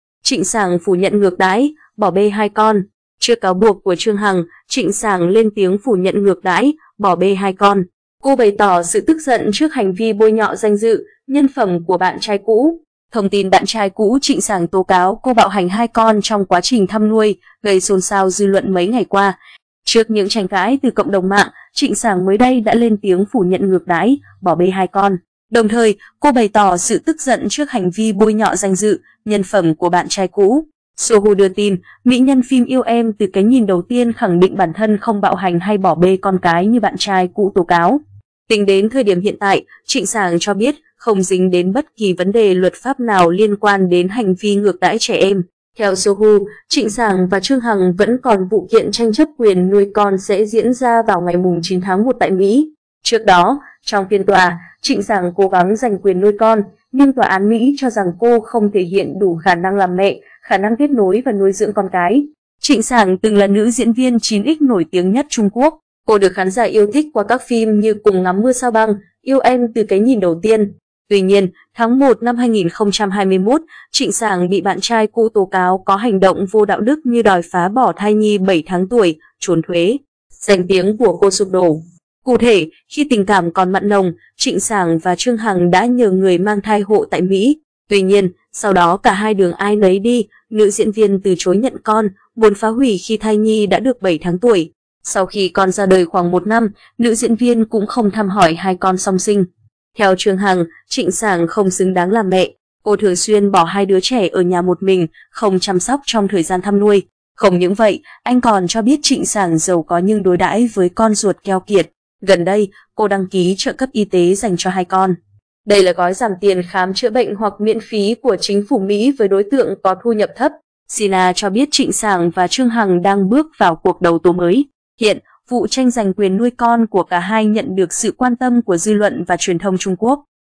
Trịnh Sảng phủ nhận ngược đãi, bỏ bê hai con
Trước cáo buộc của Trương Hằng, Trịnh Sảng lên tiếng phủ nhận ngược đãi, bỏ bê hai con. Cô bày tỏ sự tức giận trước hành vi bôi nhọ danh dự, nhân phẩm của bạn trai cũ.Thông tin bạn trai cũ Trịnh Sảng tố cáo cô bạo hành hai con trong quá trình thăm nuôi gây xôn xao dư luận mấy ngày qua. Trước những tranh cãi từ cộng đồng mạng, Trịnh Sảng mới đây đã lên tiếng phủ nhận ngược đãi, bỏ bê hai con. Đồng thời, cô bày tỏ sự tức giận trước hành vi bôi nhọ danh dự, nhân phẩm của bạn trai cũ.
Theo Sohu, Trịnh Sảng và Trương Hằng vẫn còn vụ kiện tranh chấp quyền nuôi con sẽ diễn ra vào ngày 9/1 tại Mỹ. Trước đó, trong phiên tòa, Trịnh Sảng cố gắng giành quyền nuôi con, nhưng tòa án Mỹ cho rằng cô không thể hiện đủ khả năng làm mẹ, khả năng kết nối và nuôi dưỡng con cái.
Theo Trương Hằng, Trịnh Sảng không xứng đáng làm mẹ. Cô thường xuyên bỏ hai đứa trẻ ở nhà một mình, không chăm sóc trong thời gian thăm nuôi. Không những vậy, anh còn cho biết Trịnh Sảng giàu có nhưng đối đãi với con ruột keo kiệt. Gần đây, cô đăng ký trợ cấp y tế dành cho hai con. Đây là gói giảm tiền khám chữa bệnh hoặc miễn phí của chính phủ Mỹ với đối tượng có thu nhập thấp.
Trịnh Sảng và Trương Hằng gây ồn ào showbiz Trung Quốc ngay đầu năm 2023. Ảnh: Sina.
Trịnh Sảng từng là nữ diễn viên 9X nổi tiếng nhất Trung Quốc. Cô được khán giả yêu thích qua các phim như Cùng ngắm mưa sao băng, Yêu em từ cái nhìn đầu tiên...
Trịnh Sảng lên tiếng về cáo buộc bạo hành con.
Sohu đưa tin, mỹ nhân phim “Yêu em từ cái nhìn đầu tiên” khẳng định bản thân không bạo hành hay bỏ bê con cái như bạn trai cũ tố cáo. Tính đến thời điểm hiện tại, Trịnh Sảng cho biết không dính đến bất kỳ vấn đề luật pháp nào liên quan đến hành vi ngược đãi trẻ em.
Sina cho biết Trịnh Sảng và Trương Hằng đang bước vào cuộc đấu tố mới. Hiện, vụ tranh giành quyền nuôi con của cả hai nhận được sự quan tâm của dư luận và truyền thông Trung Quốc.
Tuy nhiên, tháng 1/2021, Trịnh Sảng bị bạn trai cũ tố cáo có hành động vô đạo đức như đòi phá bỏ thai nhi 7 tháng tuổi, trốn thuế, danh tiếng của cô sụp đổ.
Cụ thể, khi tình cảm còn mặn nồng, Trịnh Sảng và Trương Hằng đã nhờ người mang thai hộ tại Mỹ. Tuy nhiên, sau đó cả hai đường ai nấy đi, nữ diễn viên từ chối nhận con, muốn phá hủy khi thai nhi đã được 7 tháng tuổi. Sau khi con ra đời khoảng một năm, nữ diễn viên cũng không thăm hỏi hai con song sinh.